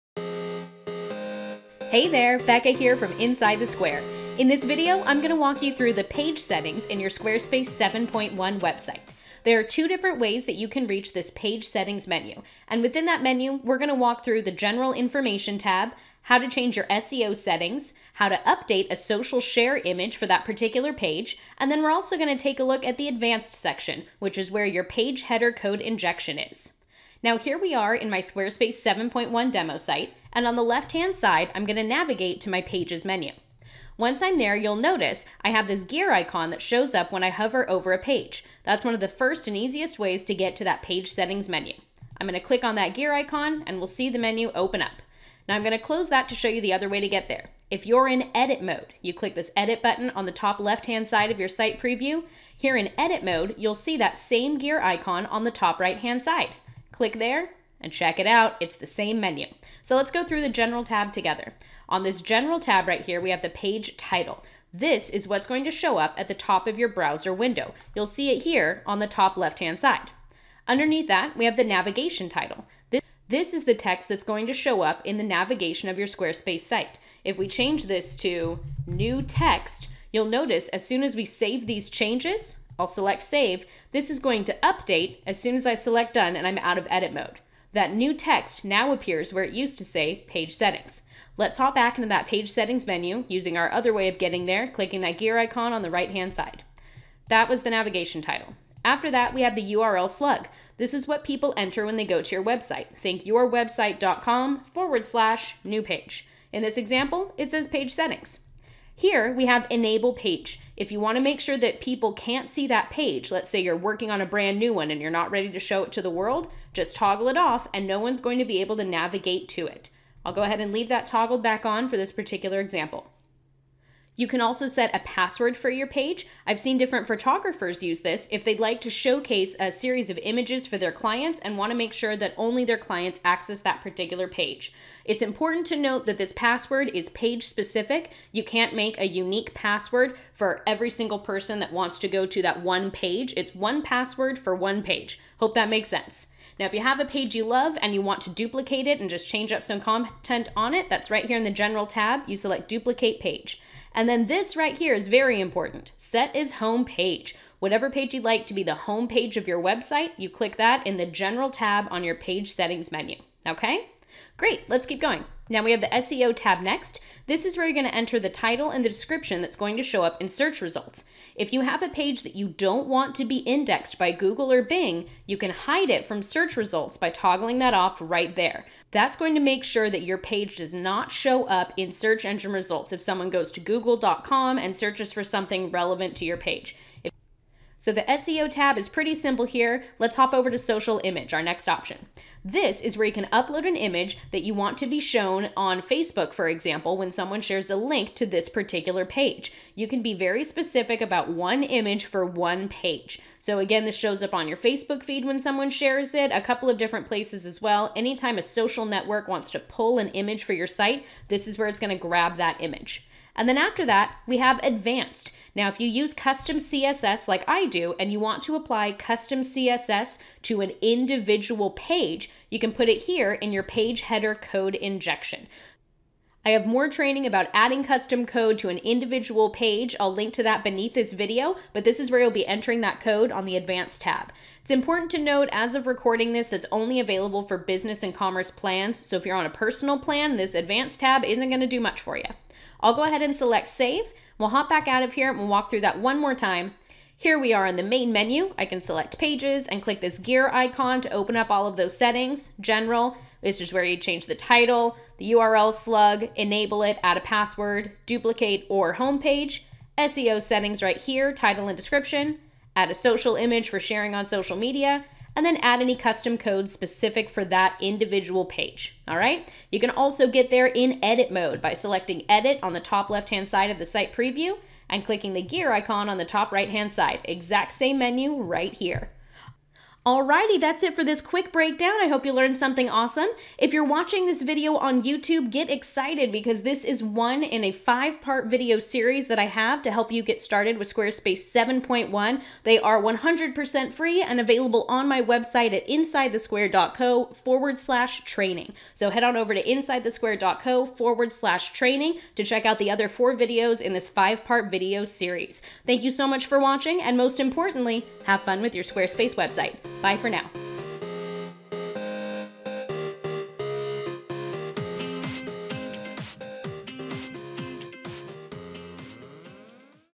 Page Settings in Squarespace 7.1 // Free Training from InsideTheSquare.co
In this video I am going to walk you through the page settings menu inside Squarespace 7.1

I’ll show you the two different ways to reach the page settings menu, and then we are going to take a look at all your options. From the general info section, we can change the page title, navigation title, URL and more. I’ll also point out where to update the SEO info for your page, how to add a social share image, and where you can add custom code to an individual page.

This tutorial is specifically for Squarespace 7.1 and doesn’t apply to Squarespace 7 sites like Brine or Bedford theme templates. If you don’t know what version you are using, check out this quick tutorial to find out